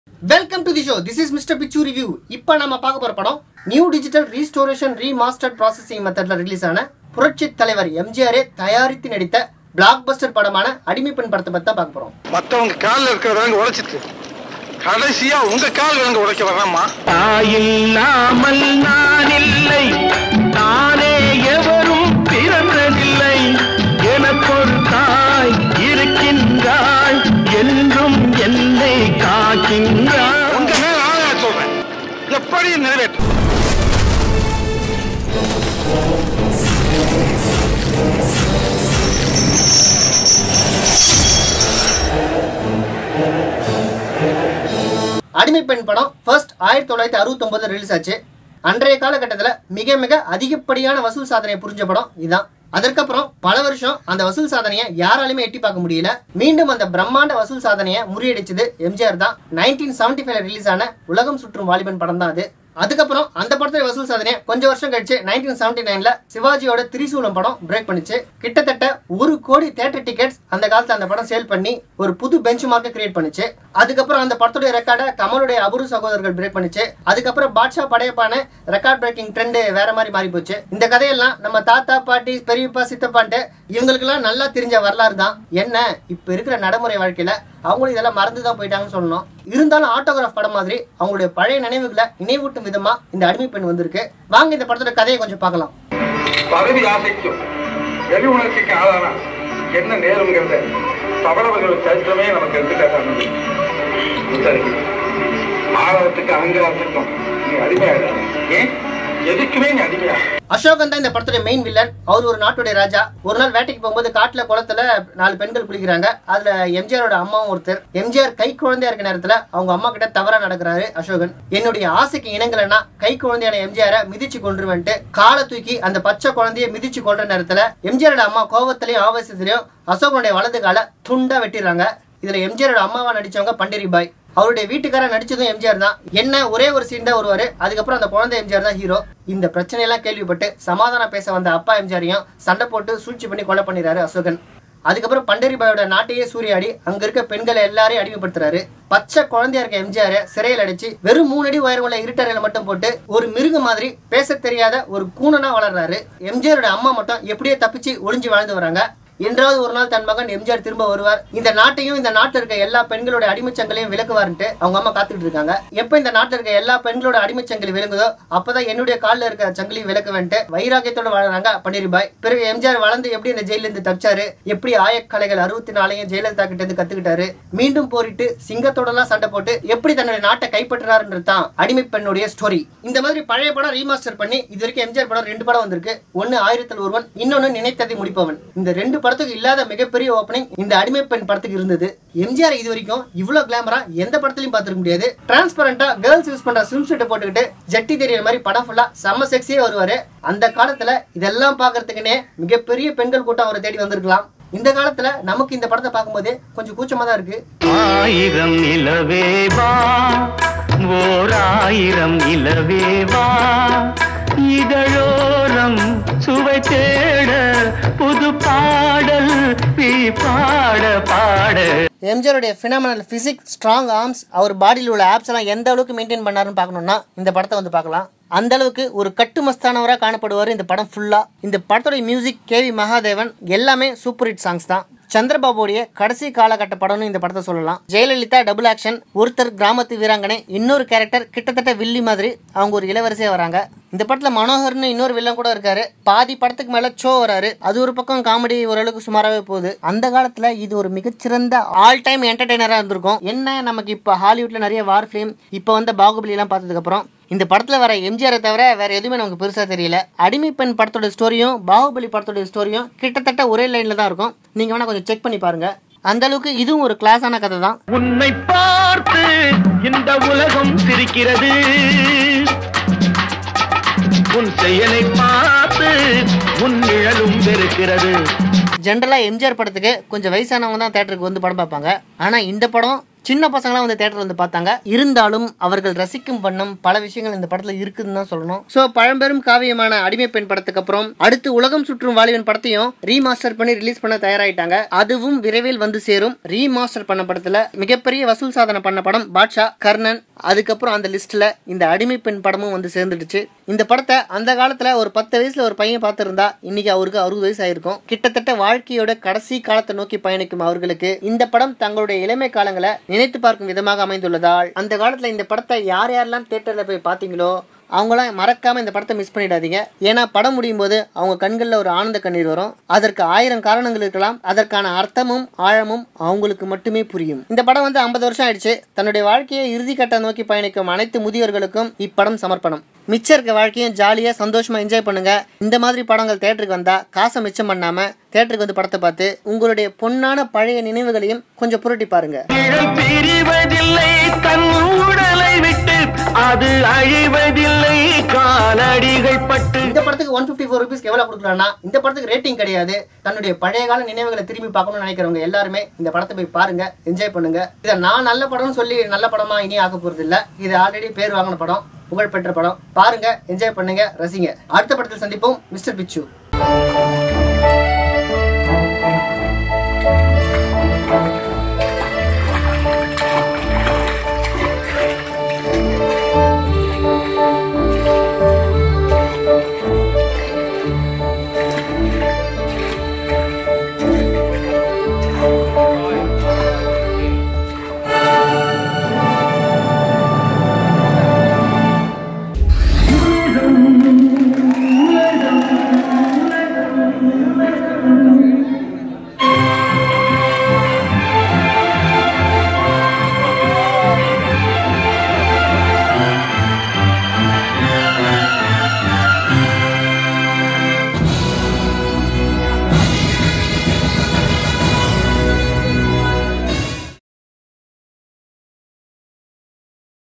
ADIMAI PENN Movie Review | அடிமைப்பெண் | MGR CLASSIC BLOCKBUSTER TAMIL FILM
சிறந்த
கதையம்சம் கொண்ட படம்...
CLASSIC BLOCKBUSTER FILM.

MOVIE REVIEW by Mr Picchu
RATING: Ticket Price: 154 -  0 = 154 Rs Only (NO% LOSS)

youtube / fb / twitter @ Mr Picchu Review